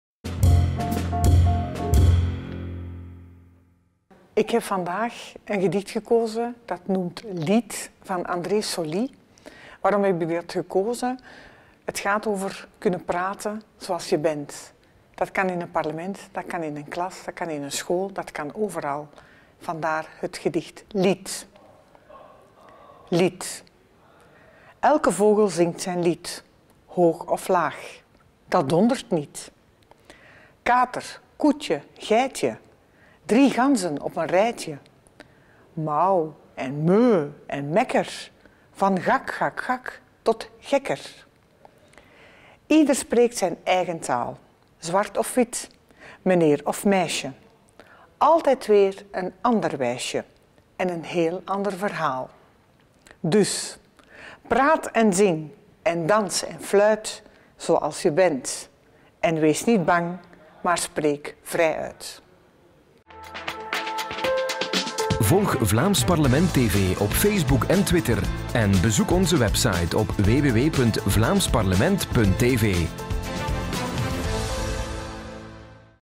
Poëzieweek 2019: Het gedicht van Ingeborg De Meulemeester
Het is een jaarlijkse traditie geworden: tijdens de Week van de Poëzie tonen parlementsleden in het Vlaams Parlement zich van hun meest lyrische kant. Poëzieweek 2019: Het gedicht van Ingeborg De Meulemeester van N-VA